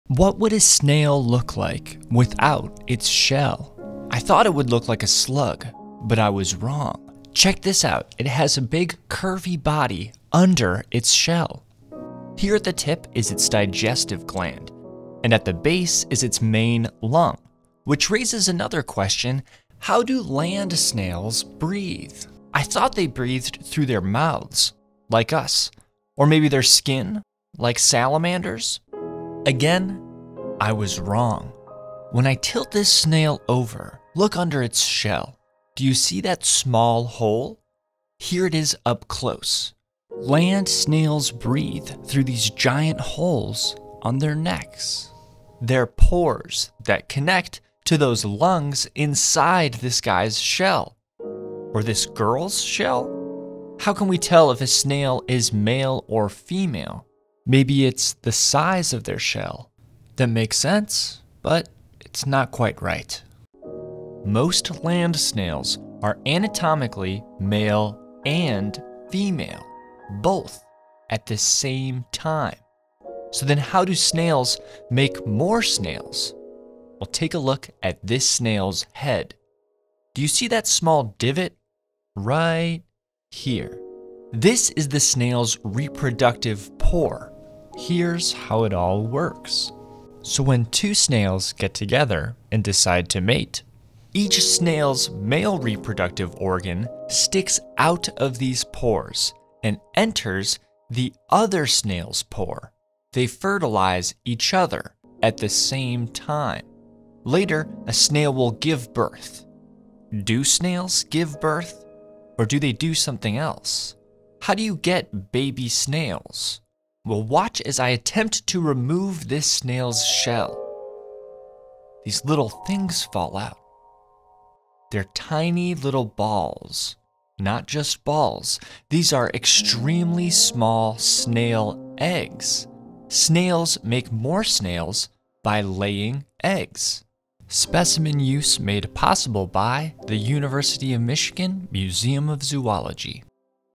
What's Inside a Snail Shell?
Here's everything you never knew about snails.
This video answers all of the following questions:
- How do snails breathe?
- How to tell if a snail is a male or female?
- Do snails give birth?
- Do snails lay eggs?